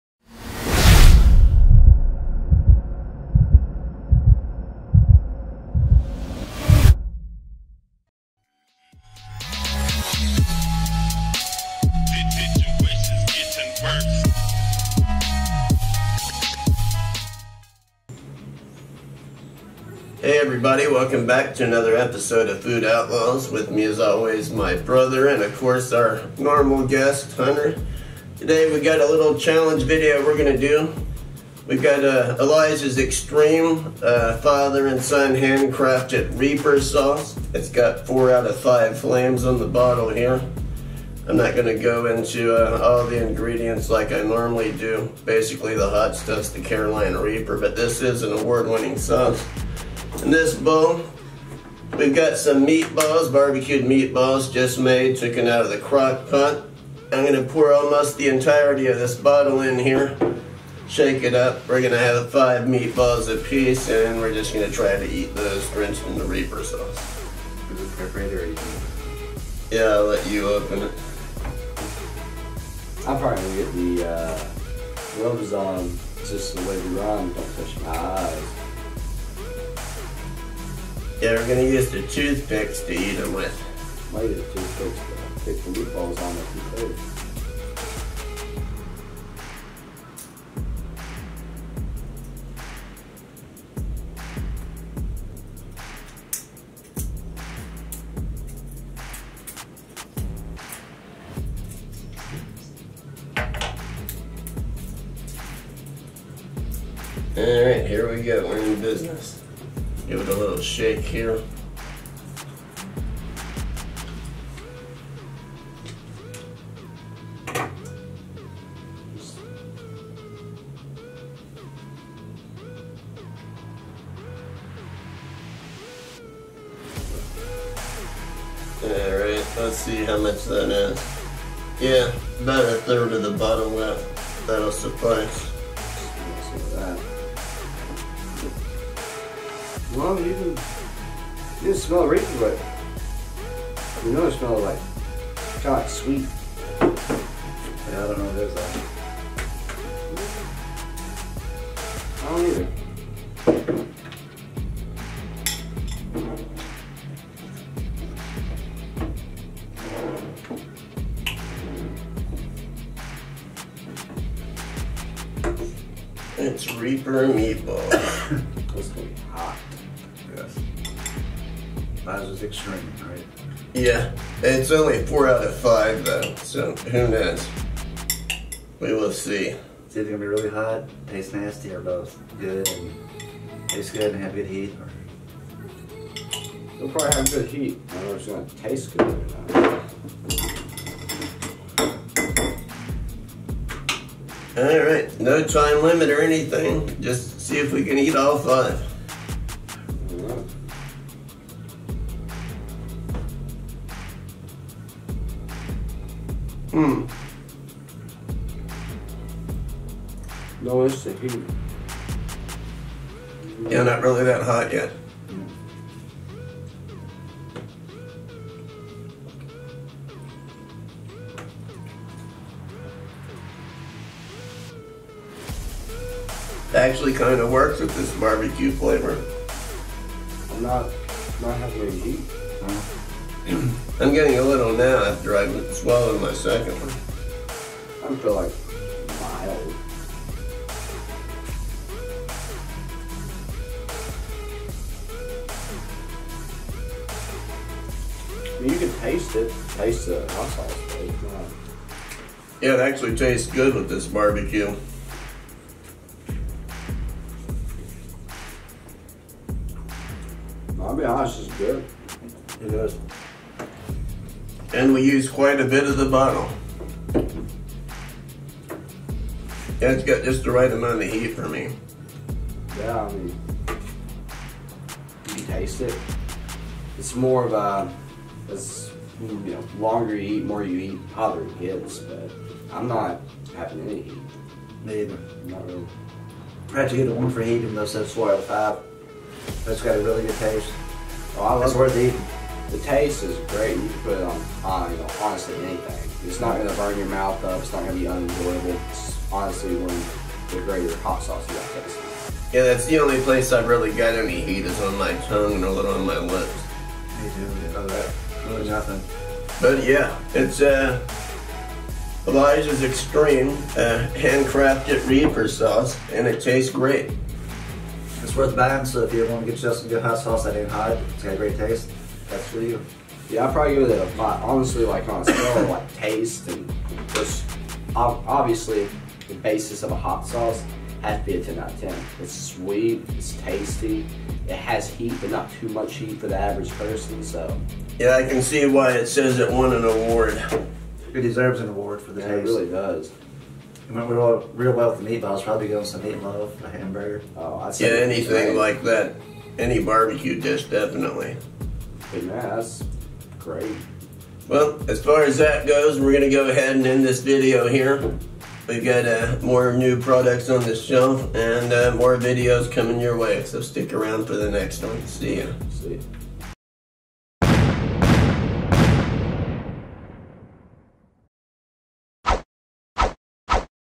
Elijah's eXtreme Reaper Sauce
In this video we preview another sauce from Elijah's eXtreme.
Send us a challenge to.
211 New Street
Horse Cave